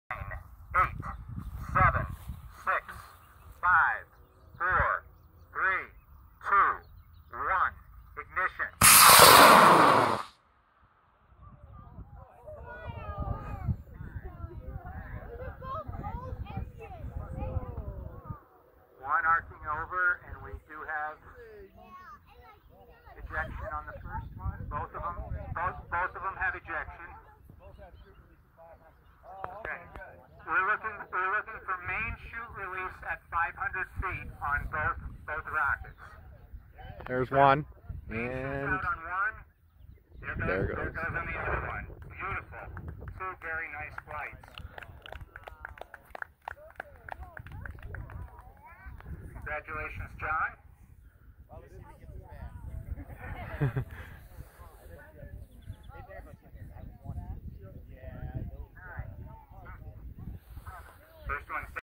Drag race of 2 Estes Mega Der Red Max Pro Series Rockets
Syracuse Rocket Club Launch 5/18/19

Neither of these are mine. The unpainted one was a level 1 certification flight.